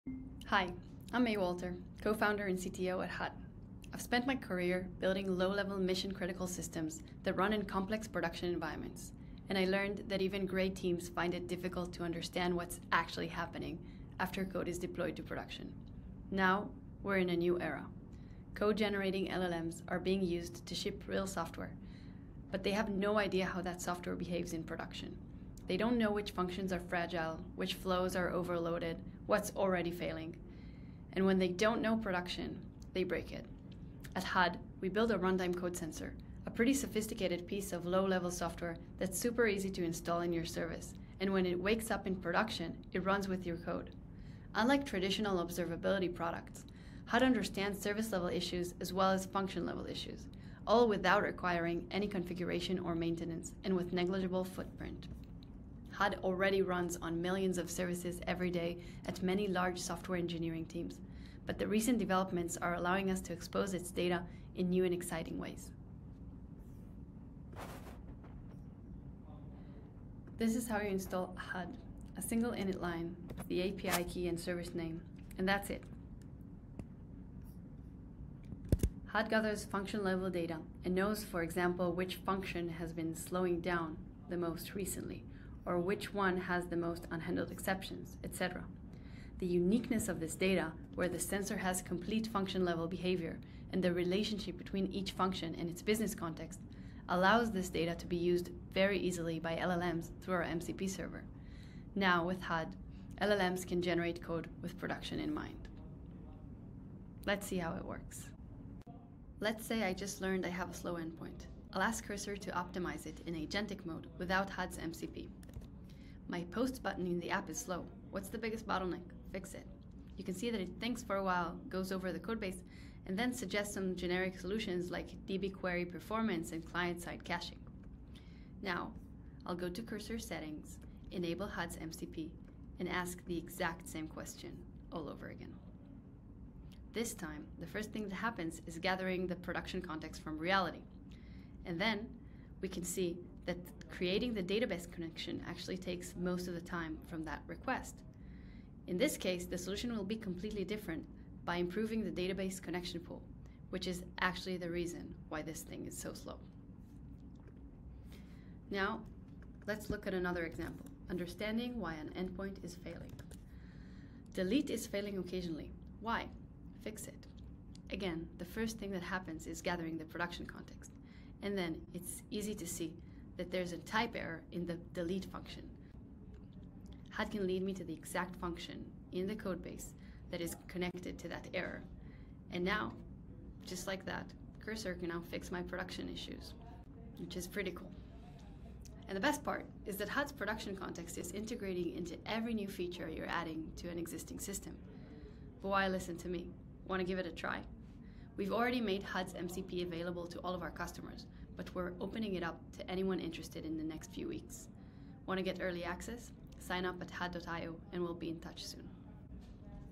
MCP for Production-Safe Code Generation using Hud’s Runtime Code Sensor
This is what it looks like when agentic AI gets access to real production data.
LLMs are now helping build real software-but without production context, they write code that often doesn’t work in the real world.
They can’t see broken flows, performance bottlenecks, or cascading failures from external dependencies-the real reasons systems go down.
Hud’s runtime code sensor changes that. It runs with your service and automatically captures function-level behavior-no configuration, instrumentation, added logs, dashboards, or maintenance needed.
With Hud’s MCP, that production context becomes available to AI environments like Cursor, Windsurf and Github CoPilot-The result? Code that’s grounded in how your system actually behaves in production.
In this video, Hud CTO May Walter walks through:
✅ How Hud runs with your code and captures real-time function behavior
✅ What makes MCP possible - and why it matters
✅ Cursor in action: same prompt, two different outcomes - with and without MCP
Once you see a model reason over live production behavior… shipping without it feels reckless.
🎥 Chapters:
00:00 – LLMs, production, and the problem we're solving
00:20 – Meet Hud’s Runtime Code Sensor
00:40 – Introducing Hud’s MCP (Model Context Protocol)
01:00 – Web + IDE: real-time function behavior
01:30 – Cursor demo: with vs. without MCP
02:30 – Production-Safe Code Generation in action